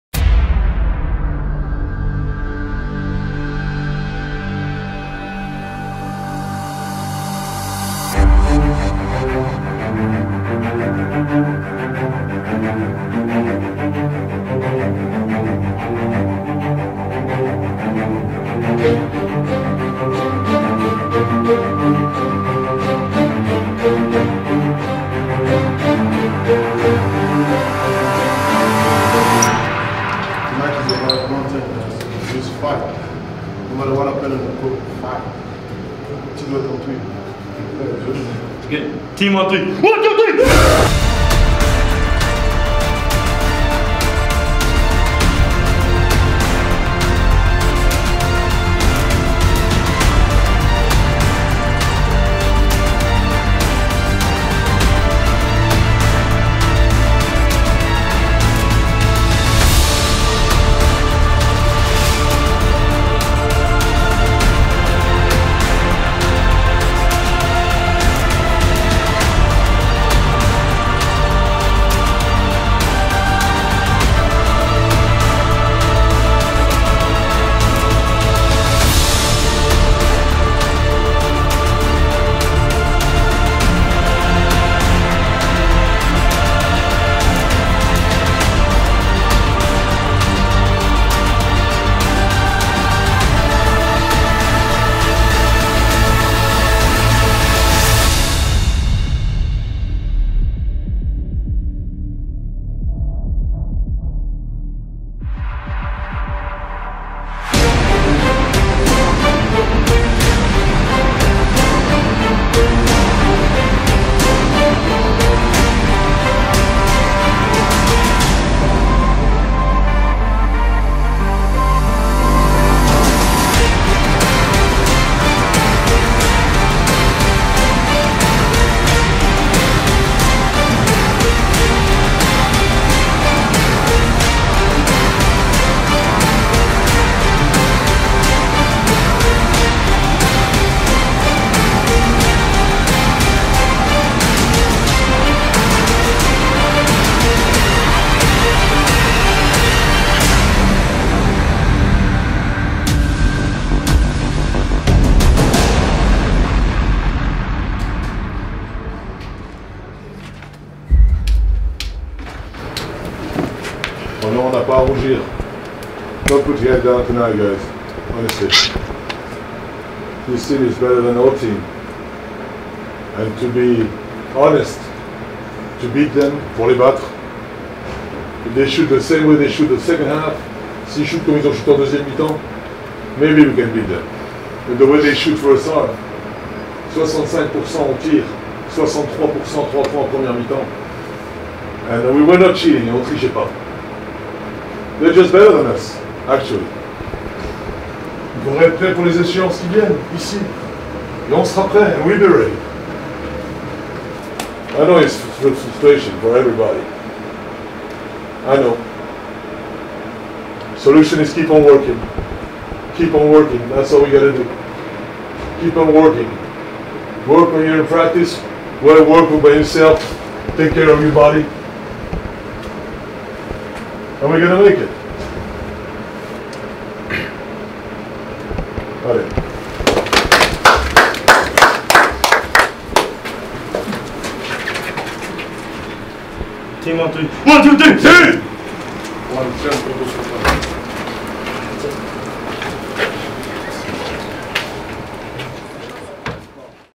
IMMERSION - Elan vs SIG
Nos joueurs se sont battus pendant 40 minutes, mais cela n’a pas suffi. Revivez ce match au plus près des acteurs.